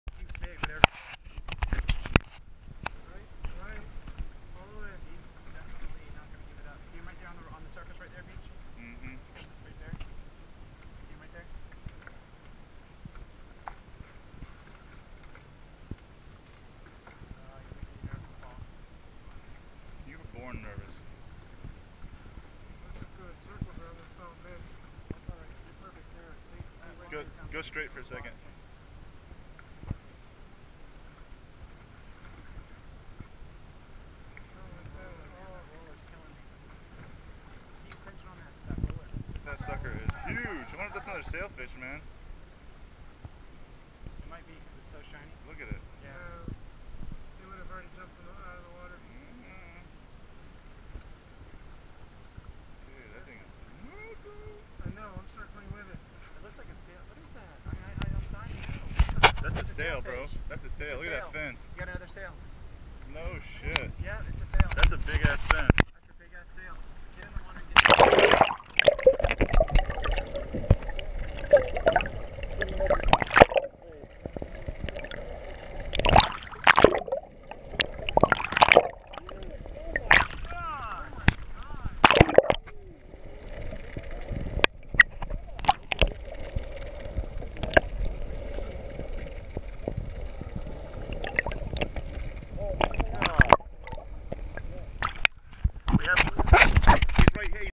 GoPro of Sailfish underwater approaching boat
While hooked up with a 30lb Bonita this Sailfish came up to the boat and I was able to reach over and grab some footage. We ended up hooking him as well, but he launched out of the water and spit our hook shortly after.